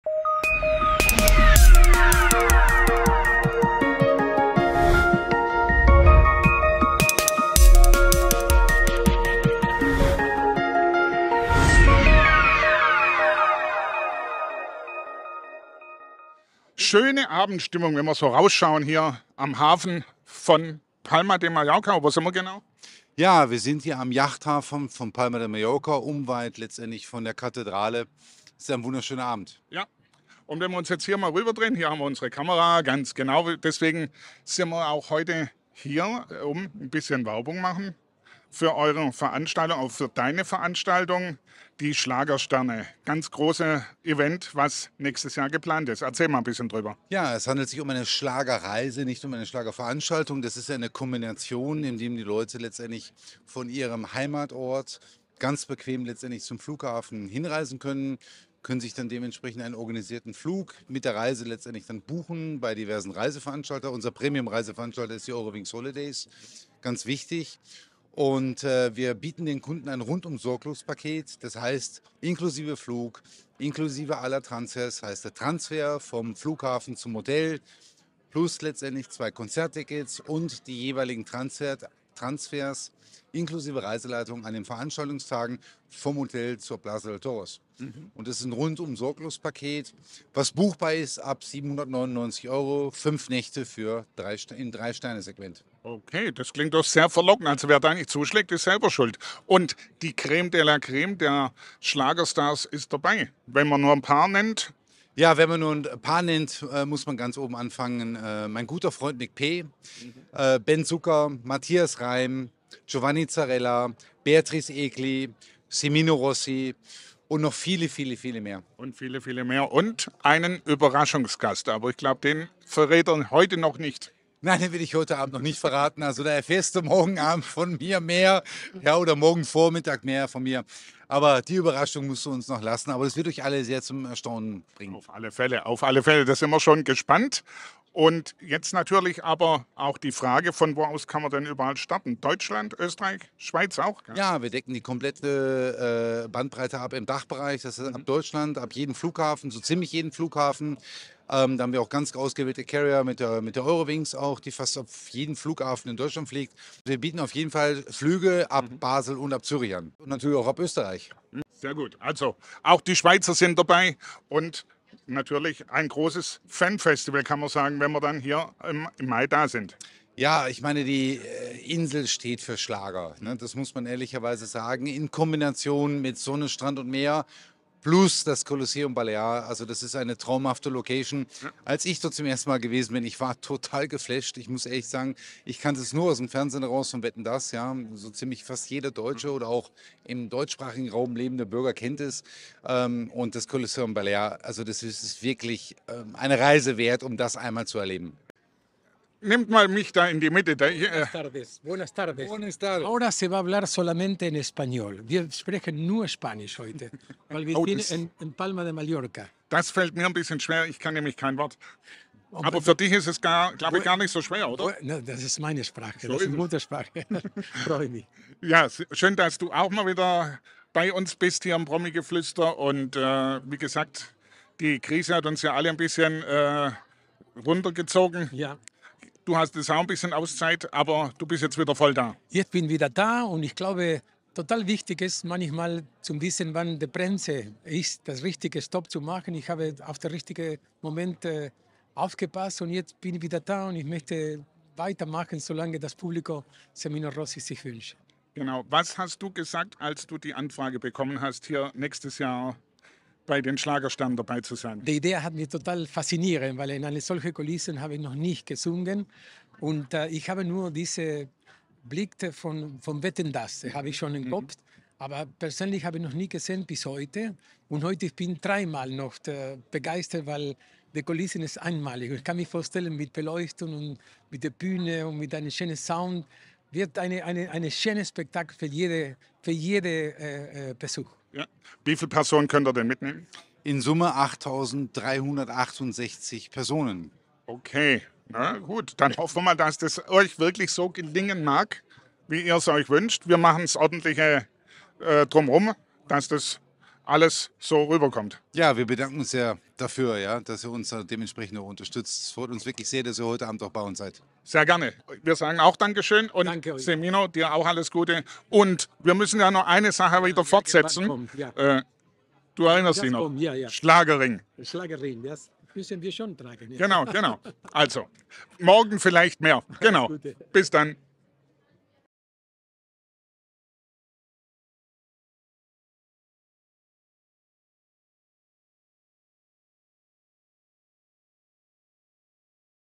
Semino Rossi - extra - startet wieder durch! Die Schlagersterne
Semino Rossi, der sympathische Argentinier fühlt sich auf Mallorca richtig wohl. Schon alleine deshalb, weil man dort die selbe Sprache spricht wie er, nämlich spanisch. Im nächsten, 2023 ist er am 10. + 11. Mai ebenfalls mit weiteren Schlagersternen auf der Bühne im Coliseo Balear und wird dort so richtig einheizen. Nach der Pandemie und persönlichen Krisen ist Semino Rossi wieder zurück auf der Bühne und freut sich. - Wie ihr bei der Reise dabei sein könnt erklärt Euch Michael Poot.